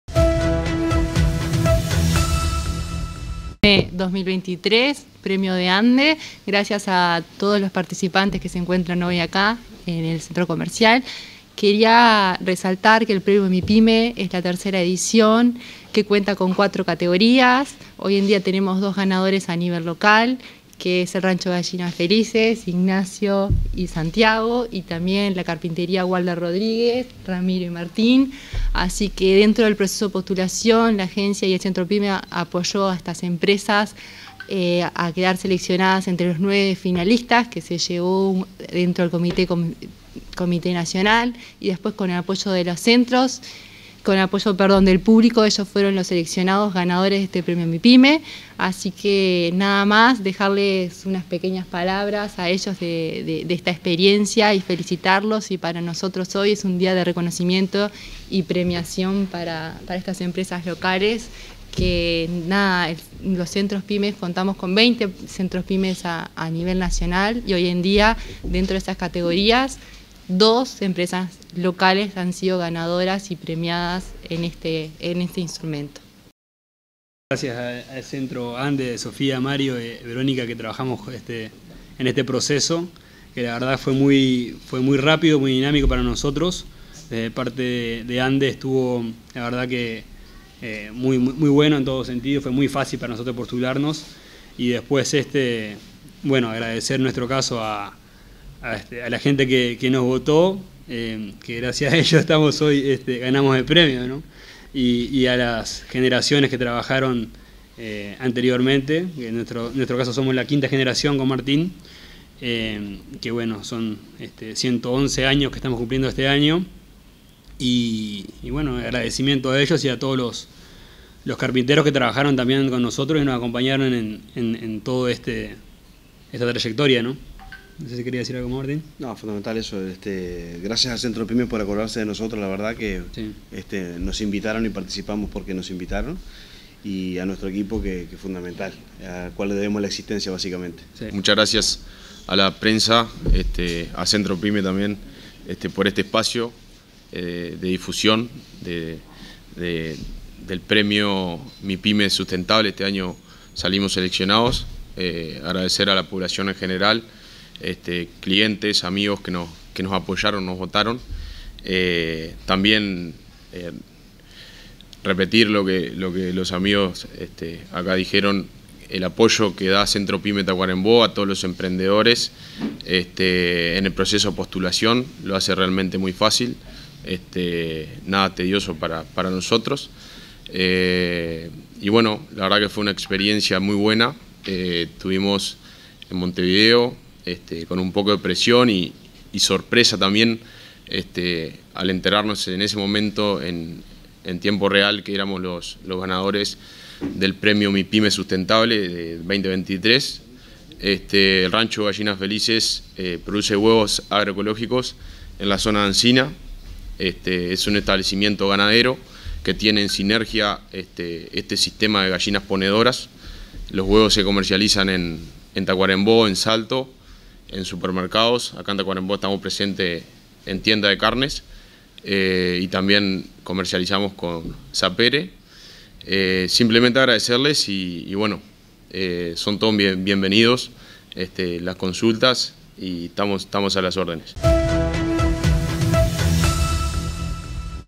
El Centro PYME de Tacuarembó presentó a los Premios Mipyme 2023
En sustentable "El Rancho Gallinas Felices" y en tradicional "Walder Rodríguez"